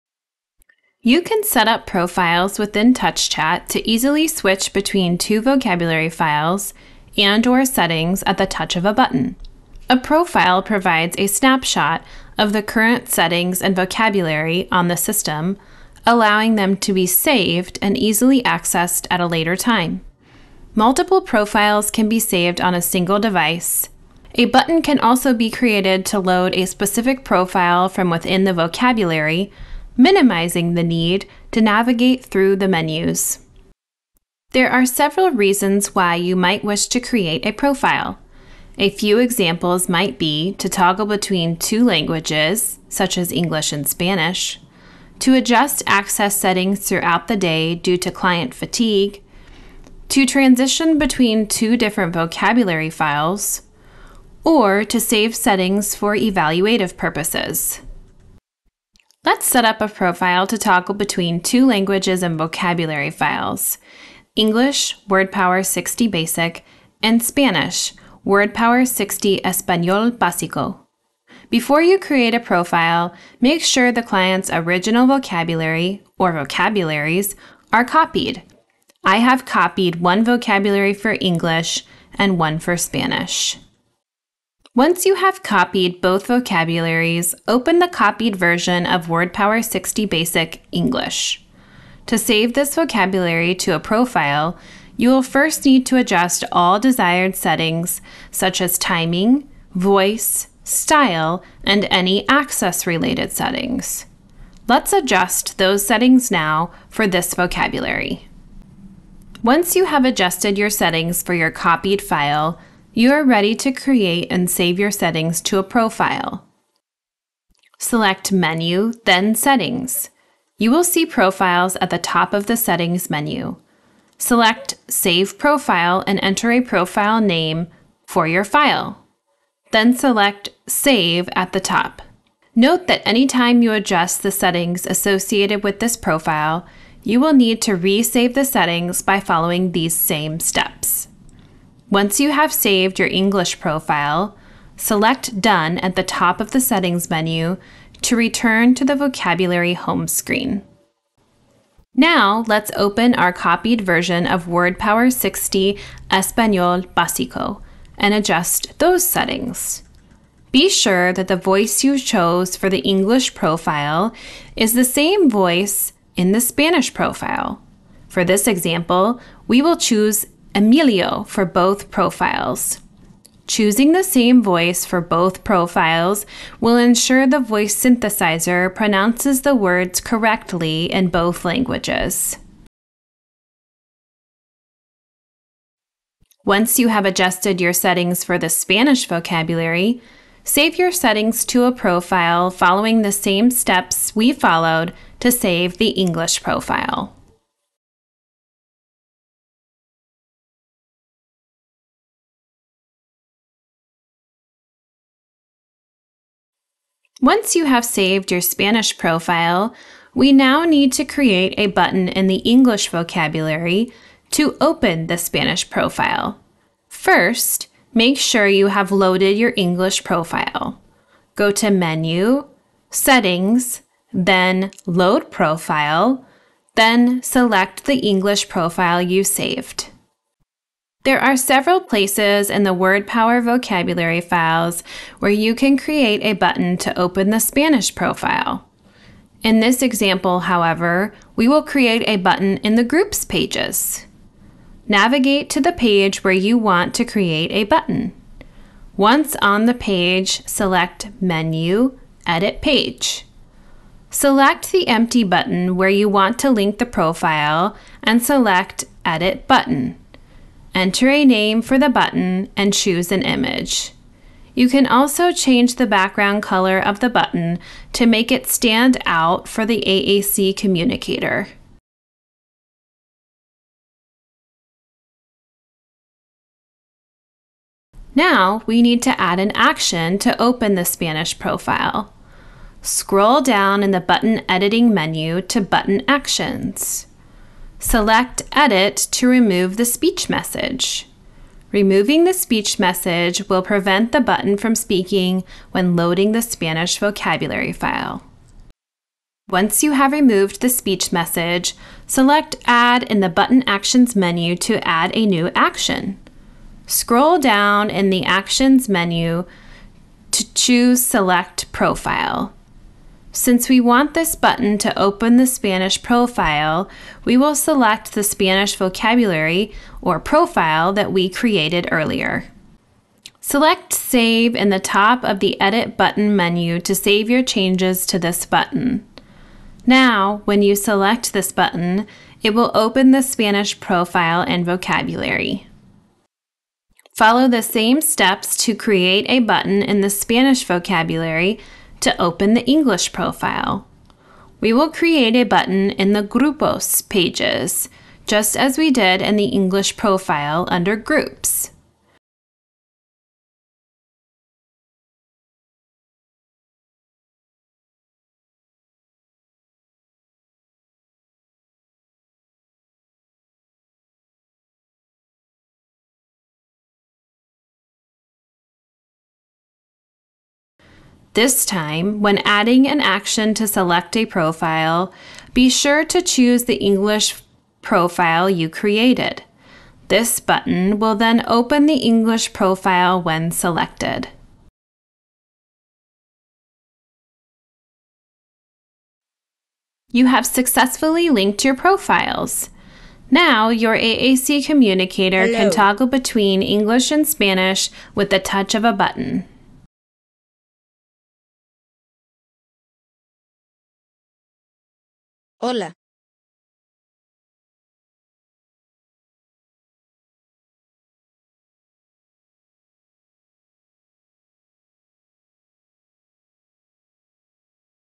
Setting up Profiles in TouchChat
Profiles are a feature available in TouchChat.  Profiles enable you to save a snapshot of an individual's settings and vocabulary.  A button action allows you to navigate easily between profiles.

This tutorial demonstrates how to create, save, load, and link profiles.

00:00-0:33- What is a Profile?
0:34-0:58-Why Profiles?
0:59-1:31-Copy vocabulary files and adjust settings
1:32-2:31-Save English Profile
2:32-3:26-Adjust Spanish settings and save Spanish Profile
3:27-5:35-Link English Profile to Spanish Profile
5:36-6:29-Link Spanish Profile to English Profile
6:30-6:58-Toggle between English and Spanish Vocabulary files